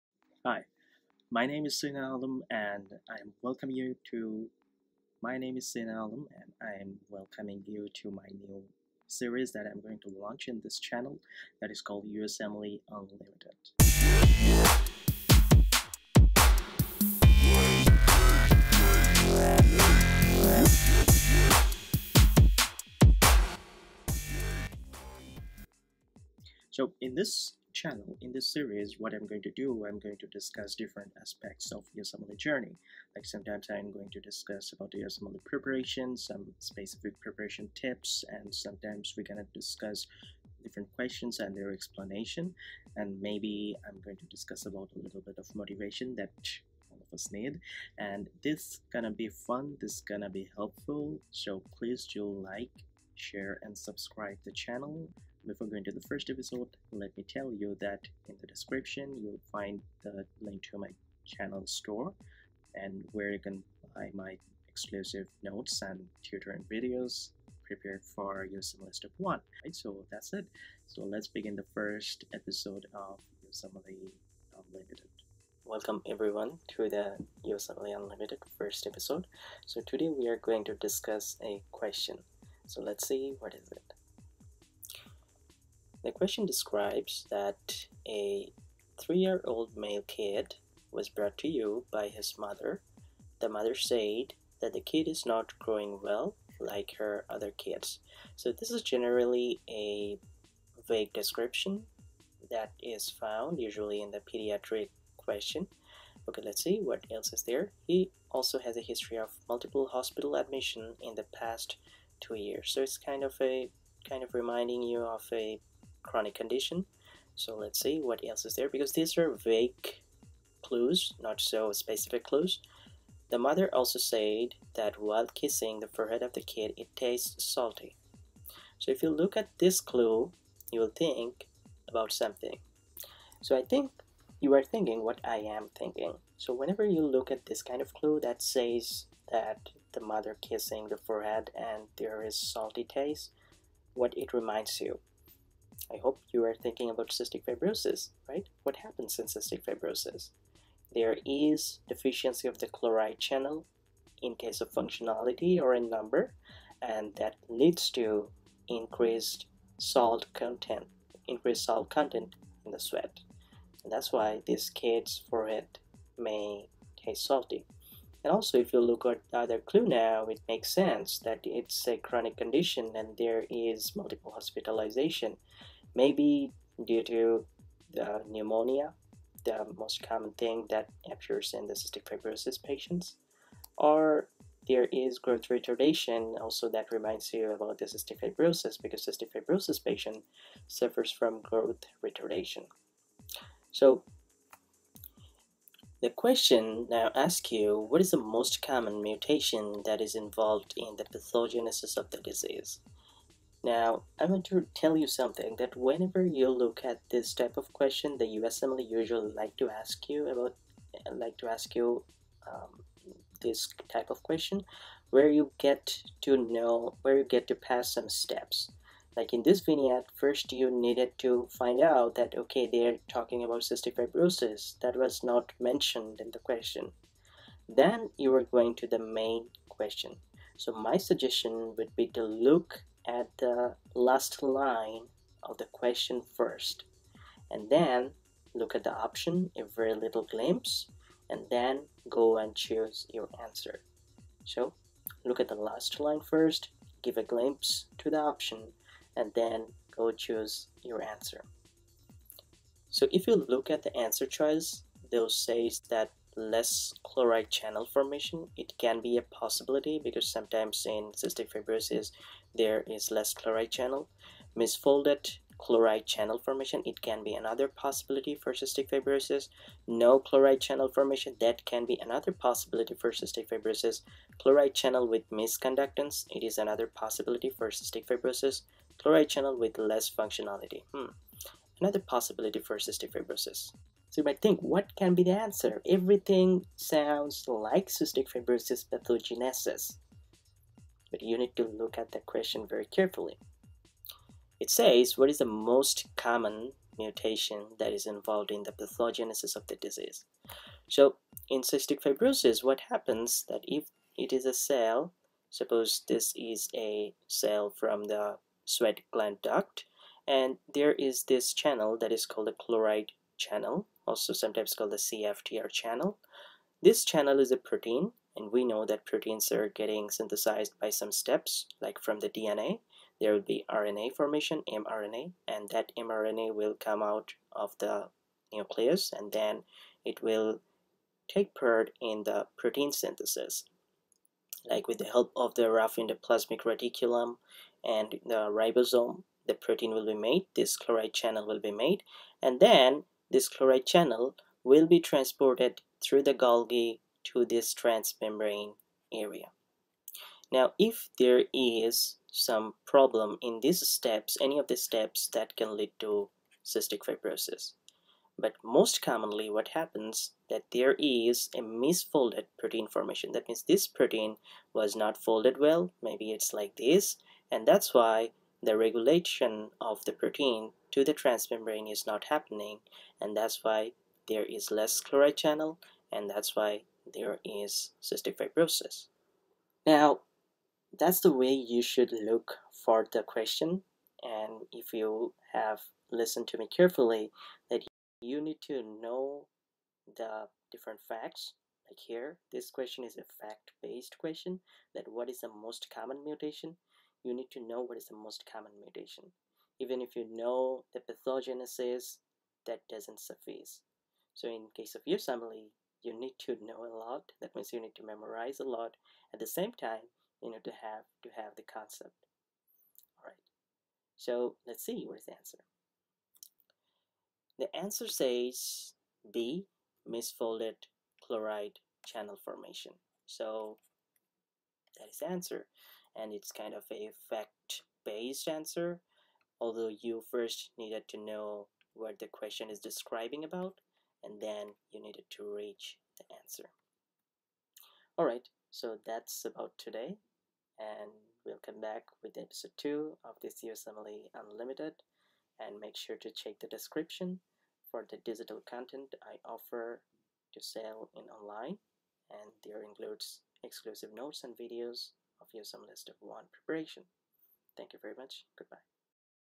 usmle-unlimited episode 1: cystic fibrosis ?
Hello Everyone!
Welcome! I am Dr Sina Alam, MD; I have been tutoring USMLE Step 1 since 2016; I have a new WhatsApp Based Tutoring program for USMLE Step 1; I talked about this program in this video with my Clone!
Only For Bangladeshis (Videos will be in Bangla here!)